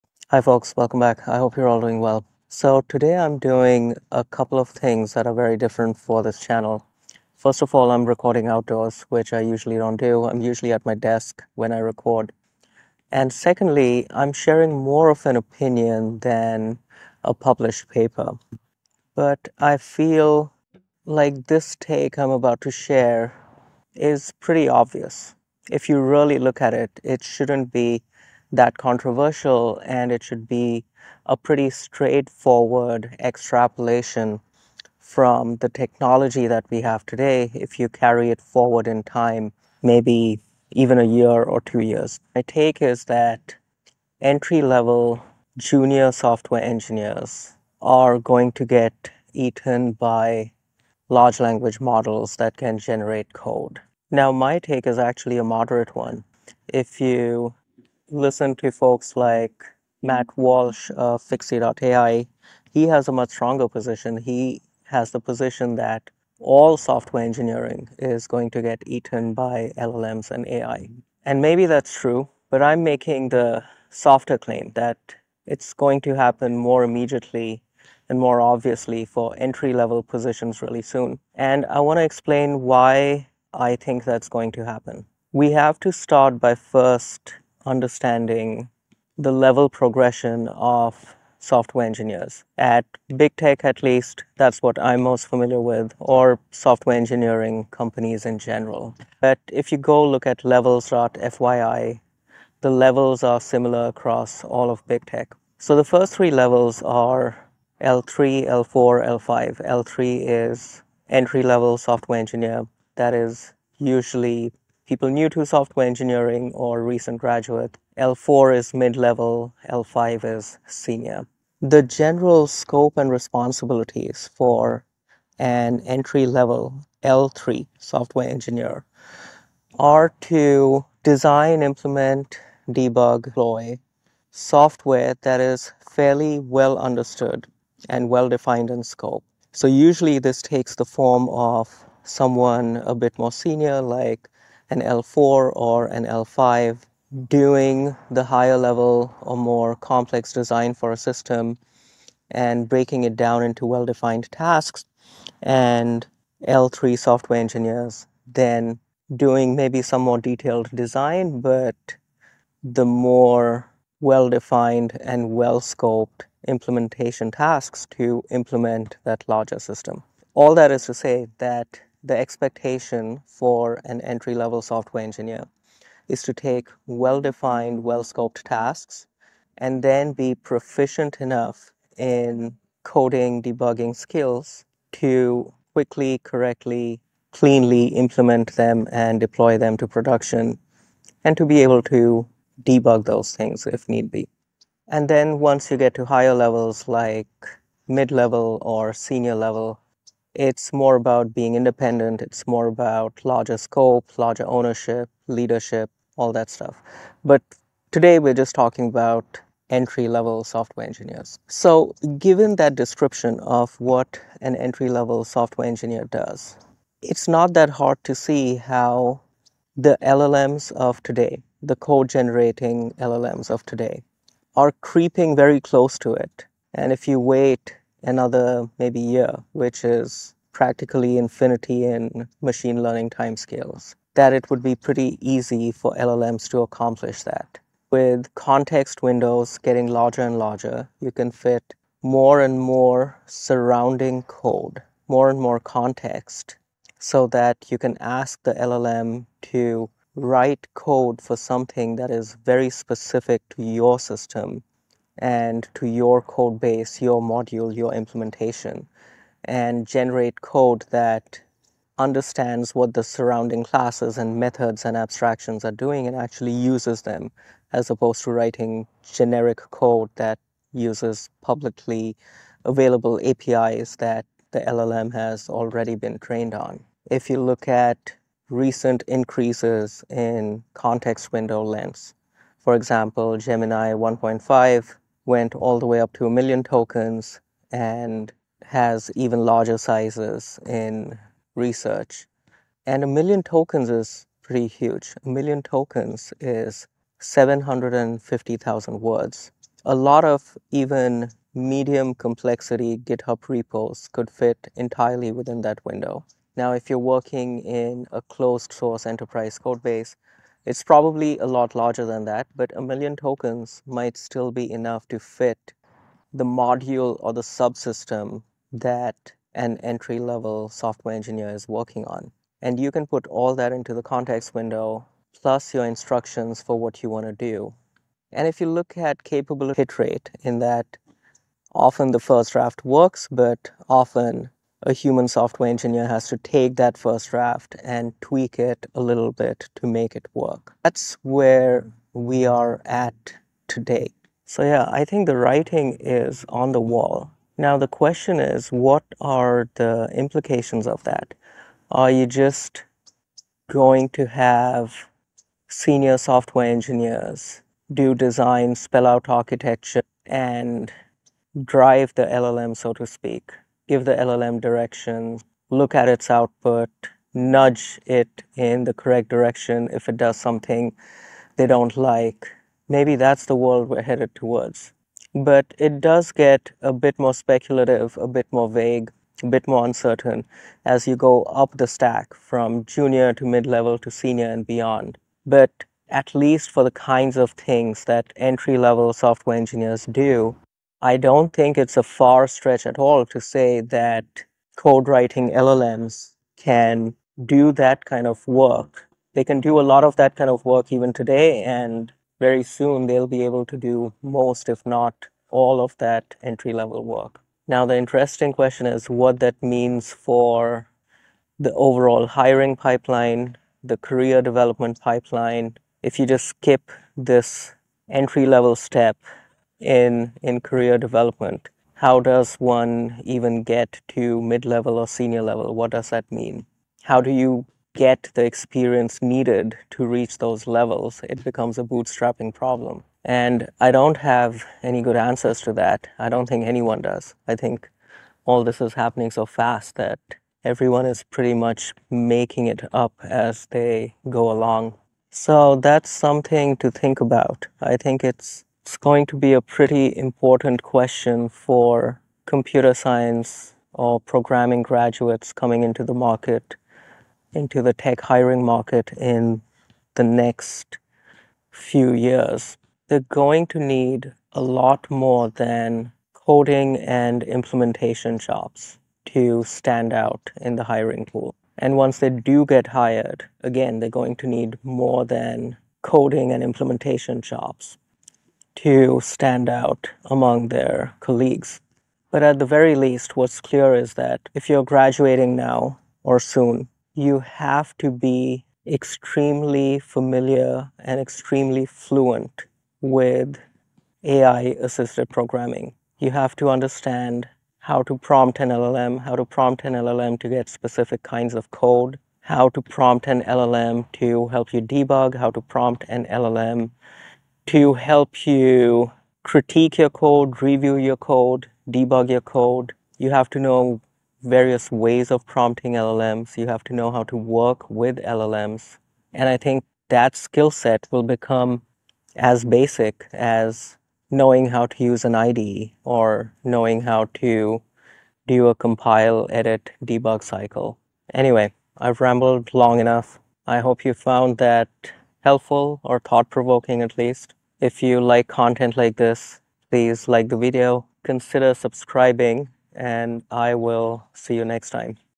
LLMs eat entry-level SWEs
00:00 Introduction and context
01:26 Software engineering levels at big tech companies
03:30 How LLMs are close to replacing entry-level engineers
05:23 Current state of LLMs and future implications
07:28 Challenges for new graduates in the tech hiring market